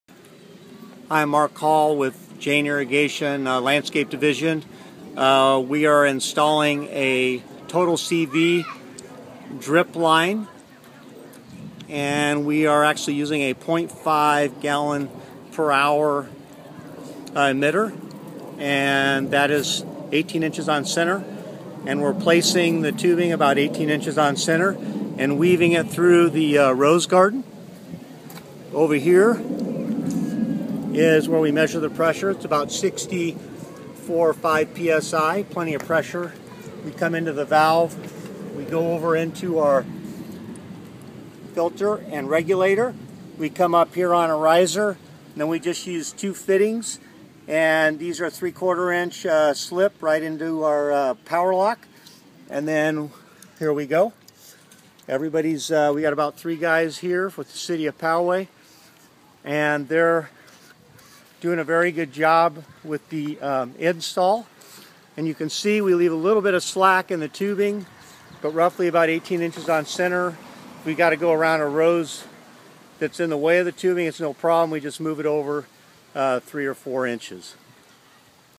Jain Total CV Installation @ City of Poway Rose Garden
750 lineal feet of .58 gal/hour emitters 18" on center w/18" row spacing.